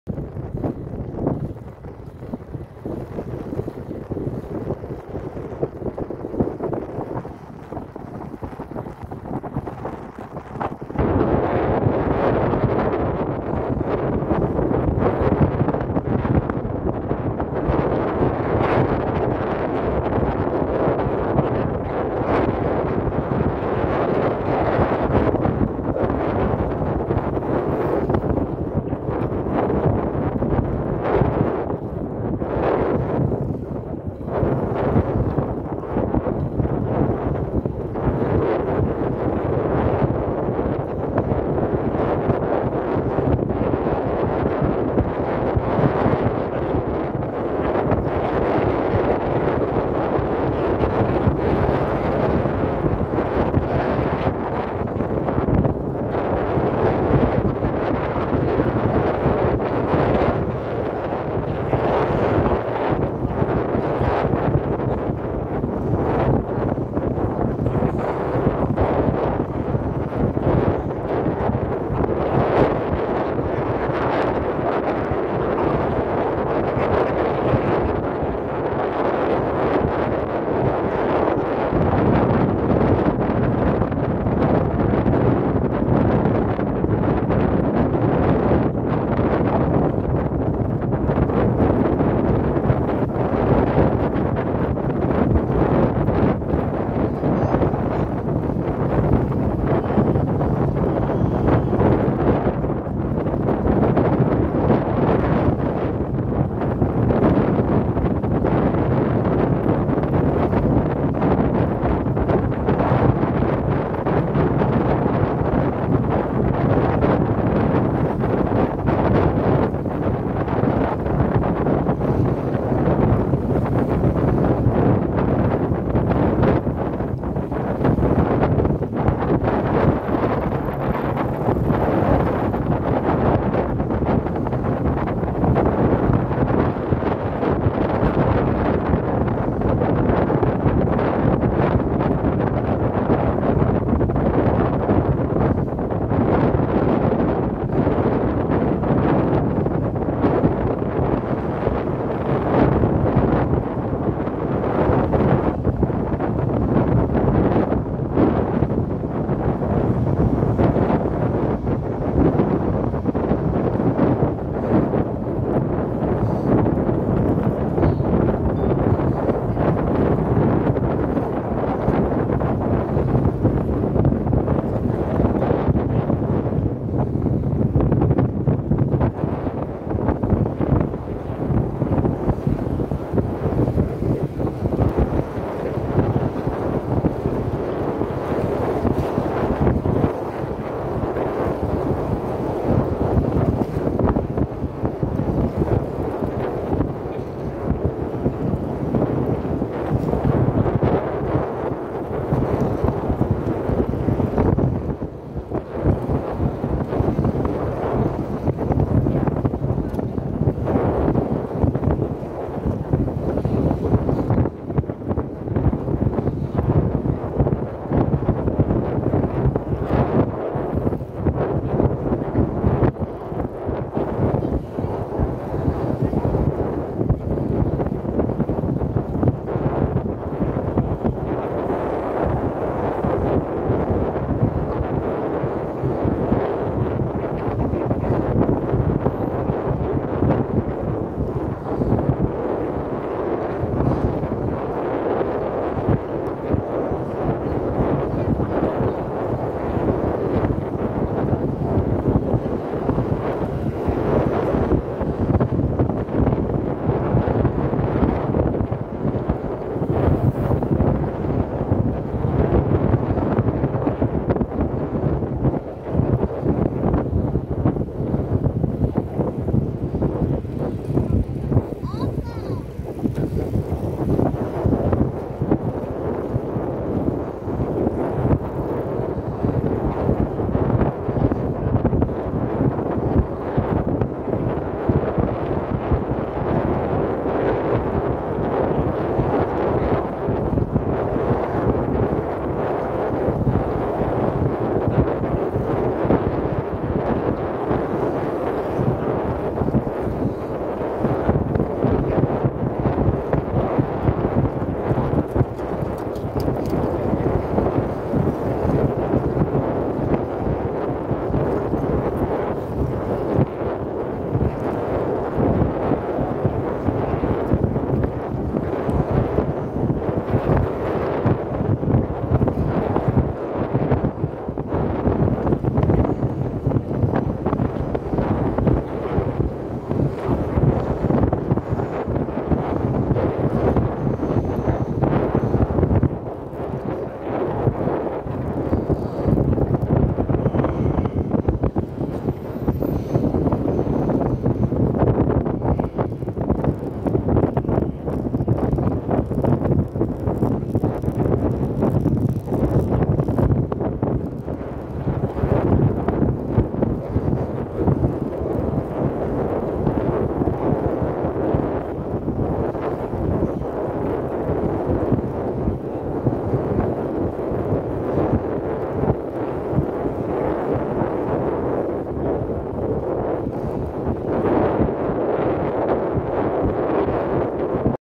Dunai áradás 2024. június
Ma tetőzött a júniusi árvíz Budapesten, épphogy elöntve a budai alsó rakpart egy kicsi szakaszát a Margit híd és a Lánchíd közötti szakaszon.
A felvétel a Margit híd és a Lánchíd között készült.